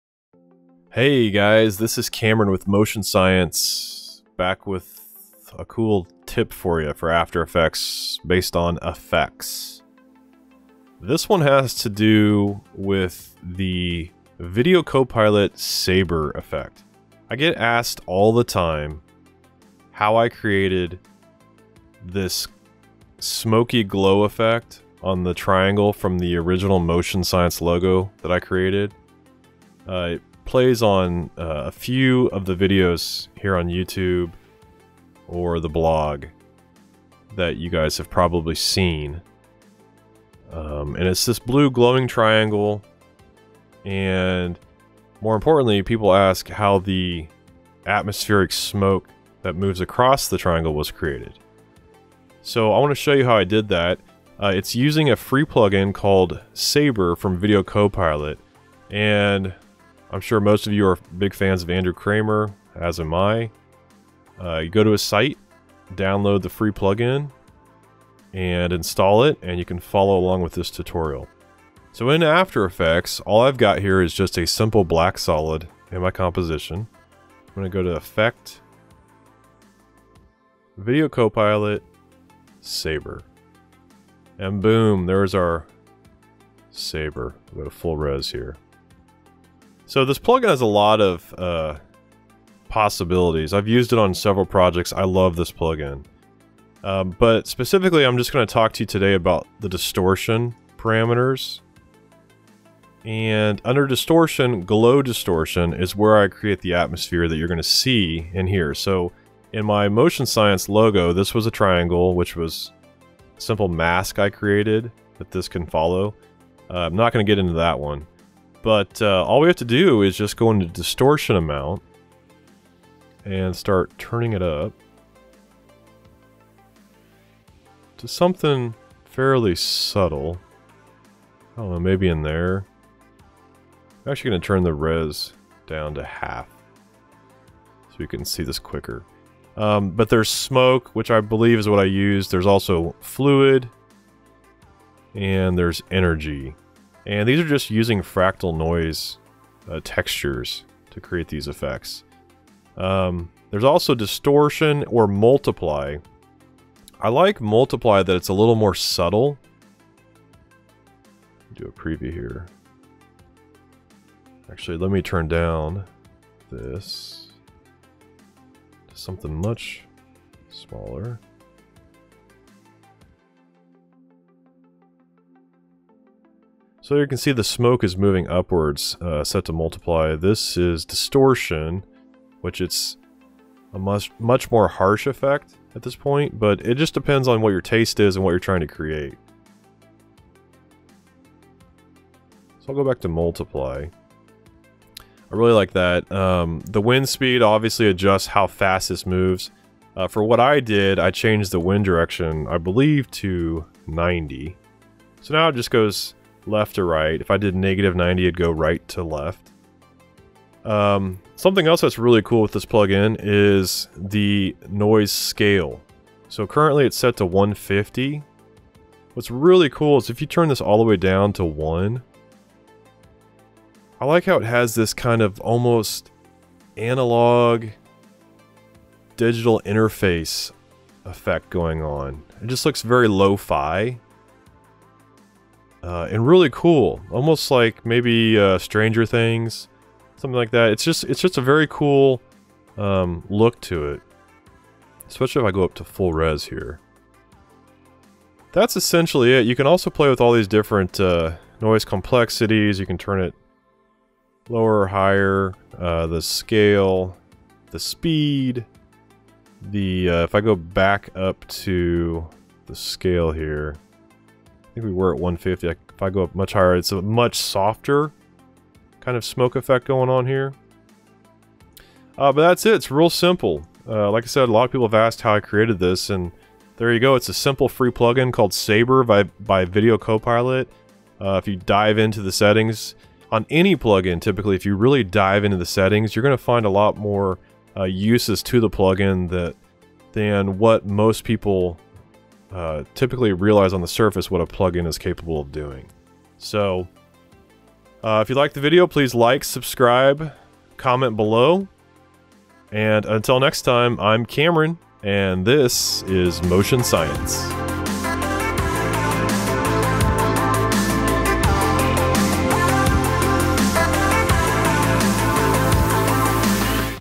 How to easily create a smoky atmosphere using Saber's distortion
In this video learn about the power of a simple distortion effect from Video Copilot. With this free effect you can easily add smoky atmosphere to any scene.